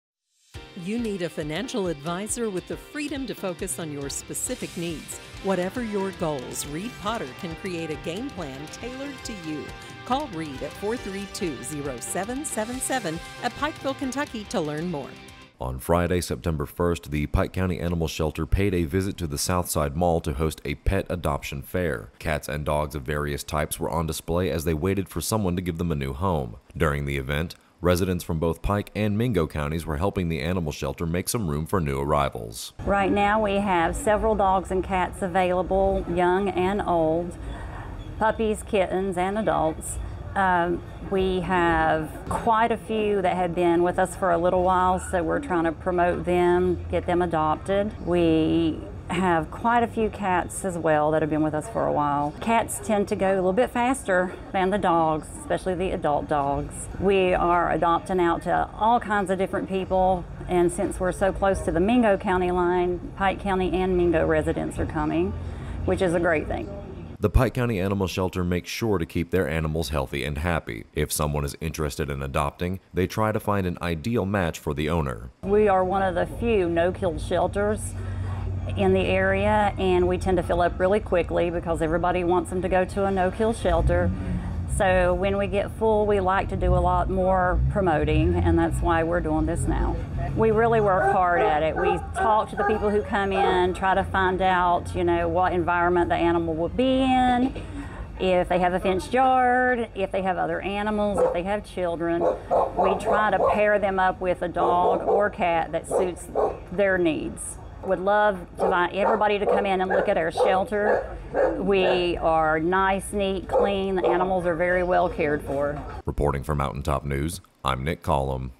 Pet Adoption Fair in South Williamson Gives Animals a Chance to Find New Homes
The Pike County Animal Shelter held a pet adoption fair at the Southside Mall in South Williamson, Kentucky, on Friday, September 1.